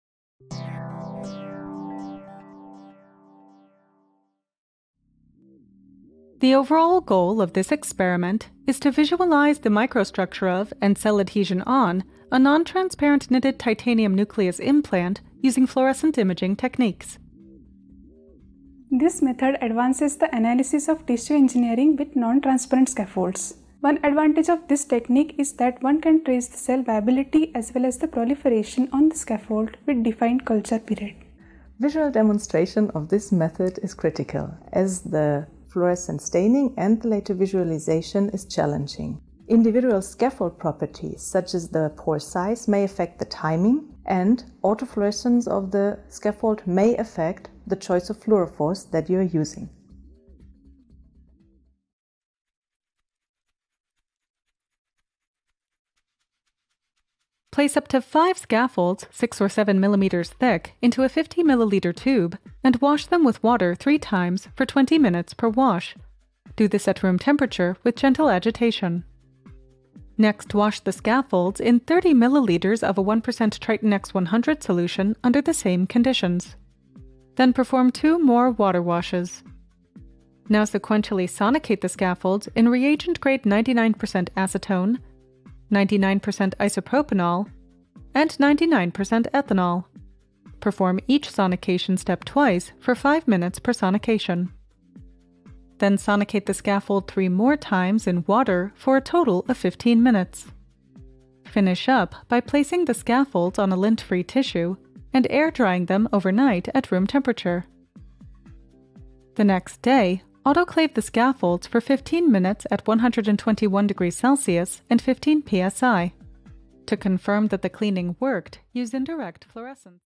Imaging Cell Viability On Non-transparent Scaffolds-Preview
Imaging Cell Viability on Non-transparent Scaffolds — Using the Example of a Novel Knitted Titanium Implant -  a 2 minute Preview of the Experimental Protocol

Gauri Tendulkar, Phillip Grau, Patrick Ziegler, Alfred Buck, Sr., Alfred Buck, Jr., Andreas Badke, Hans-Peter Kaps, Sabrina Ehnert, Andreas K. Nussler
Eberhard Karls Universität Tübingen, Siegfried Weller Institute for Trauma Research at the BG Trauma Center; BG Trauma-Center, Department of Orthopaedics; Buck GmbH and Co.KG,;

Here we present a fluorophore based imaging technique to detect cell viability on a non-transparent titanium scaffold as well as to detect glimpses of the scaffold impurities. This protocol troubleshoots the drawback of imaging cell-cell or cell-metal interactions on non-transparent scaffolds.